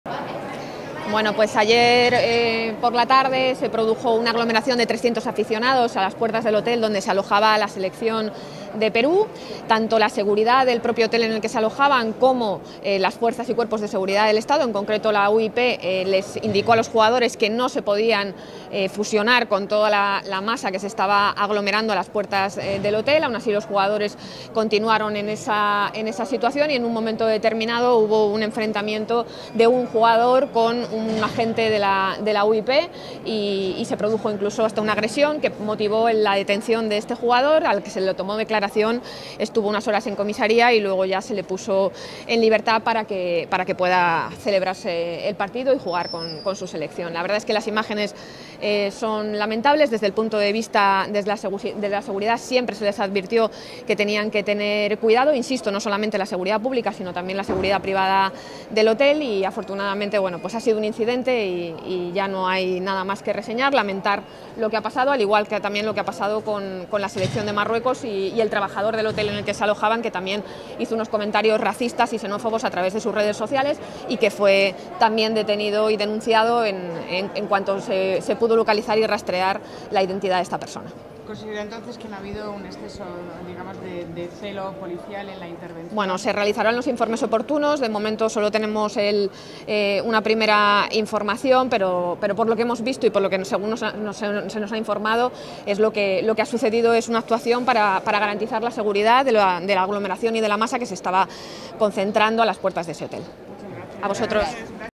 Delegada Gobierno respalda la actuación policial en incidentes con jugadores del Perú
La delegada del Gobierno en Madrid y futura directora de la Guardia Civil, Mercedes González, ha respaldado esta mañana la intervención policial en los incidentes producidos anoche a las puertas del hotel donde se alojaban los juzgadores de la selección de fútbol de Perú.